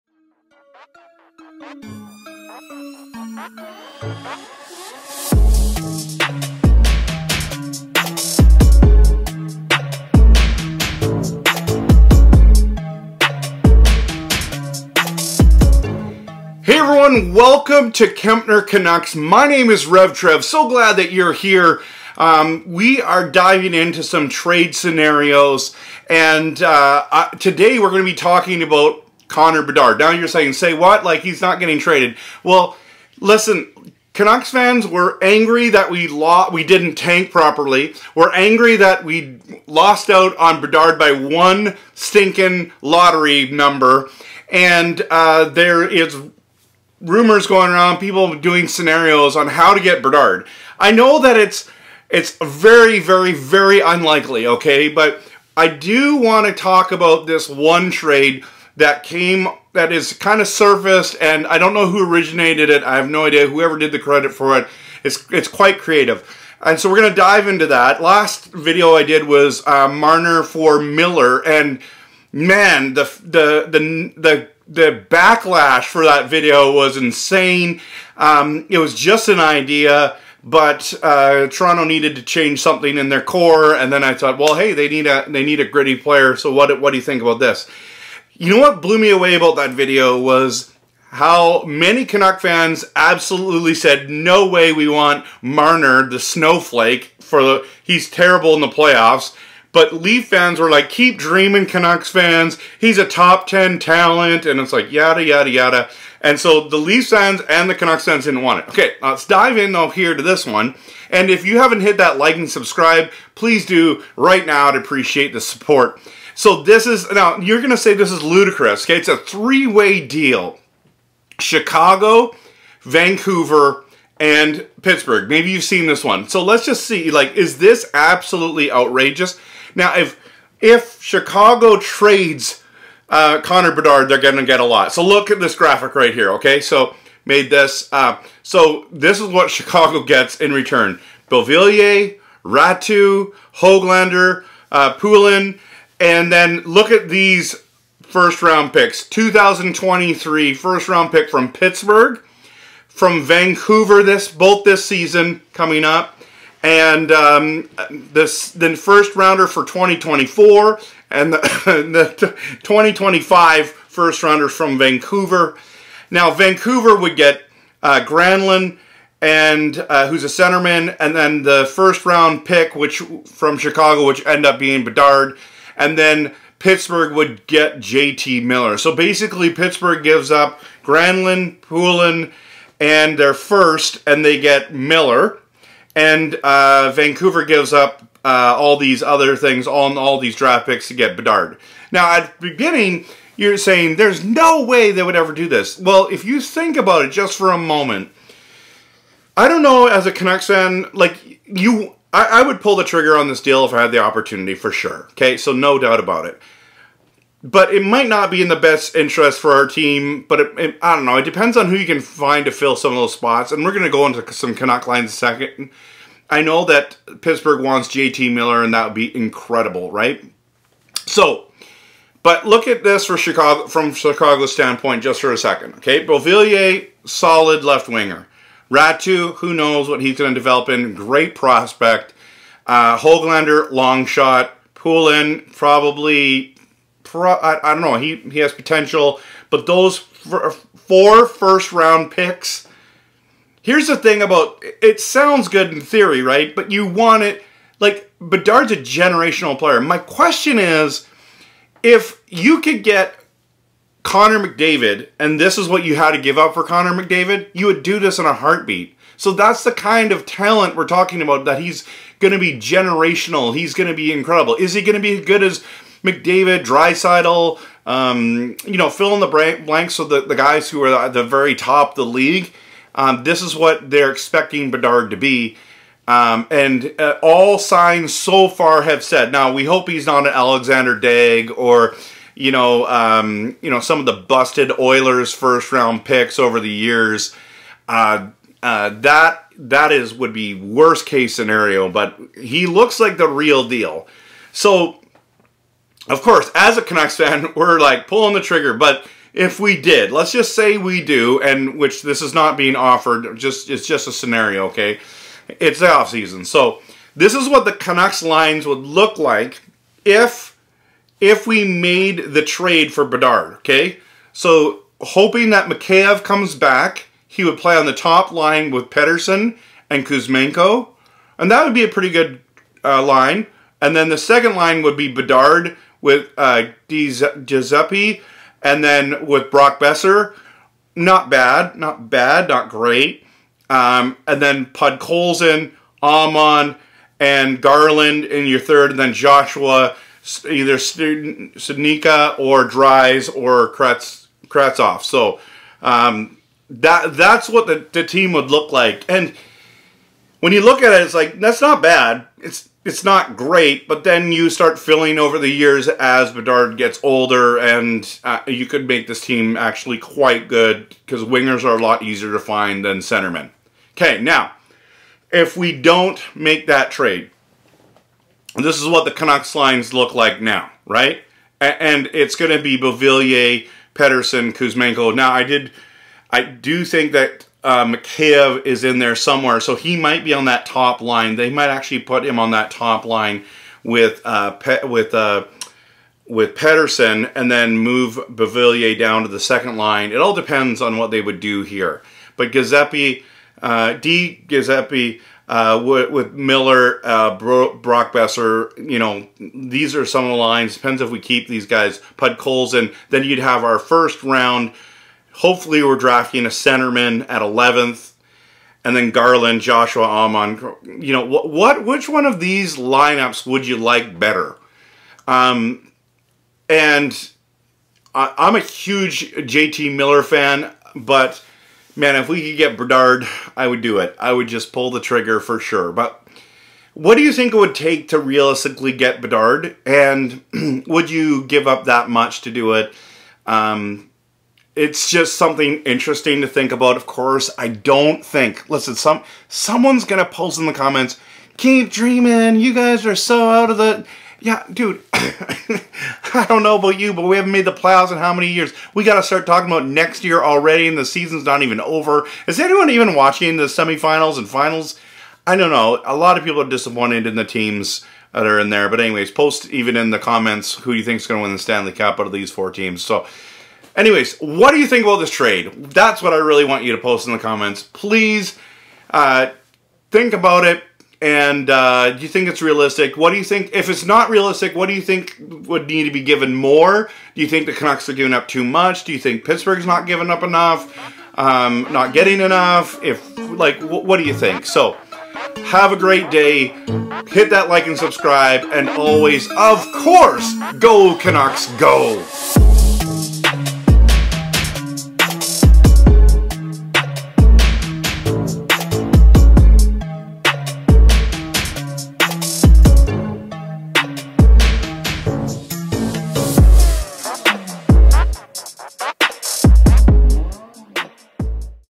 3 Team Bedard Trade Canucks, Penguins, & Blackhawks
Here's my take on the made up 3 team Connor Bedard trade involving the Blackhawks, Canucks, & Penguins.